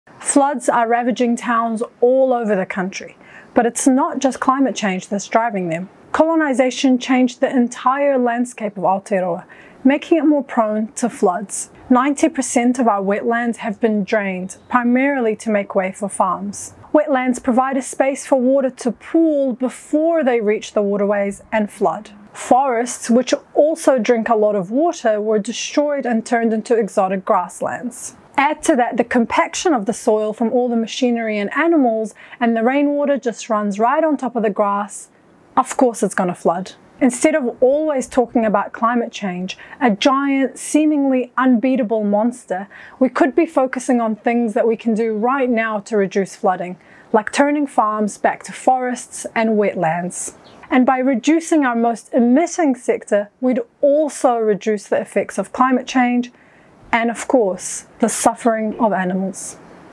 What causes floods?
Of course cities have problems of over-development, but most floods happen in rural areas. Here's why.

Kaikōrero: Samah Seger